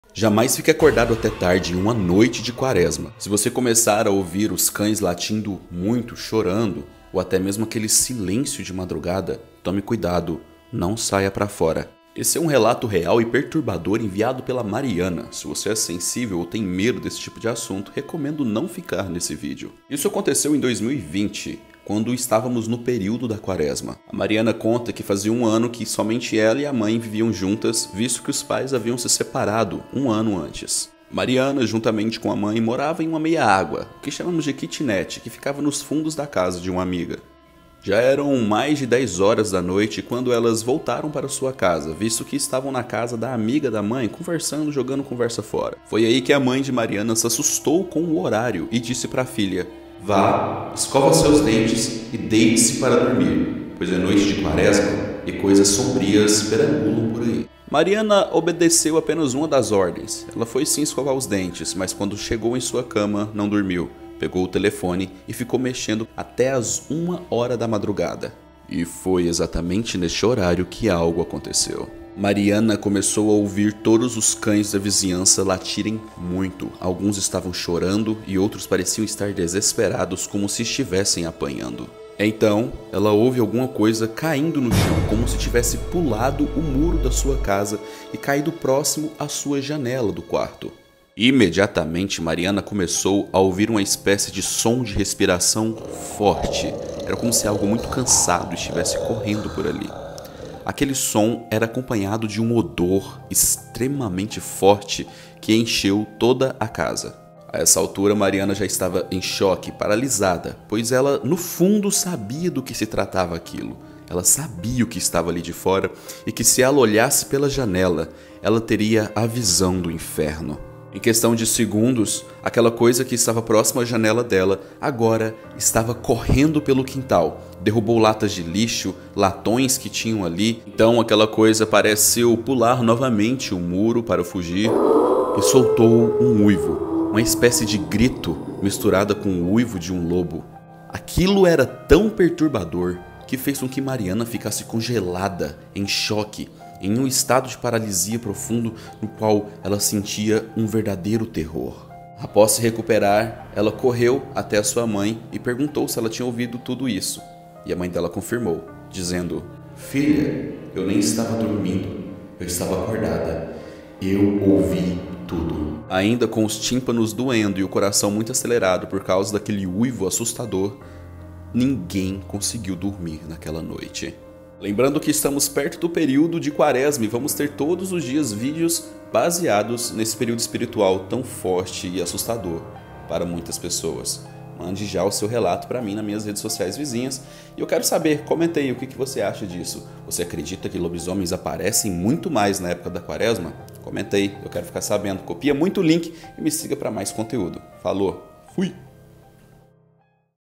Lobisomem invade casa de seguidora na quaresma - Relatos com David Lima
Mande seu relato bem detalhado em forma de texto, e ele terá mais chances de ser contado aqui. Valeu família de assombraditos !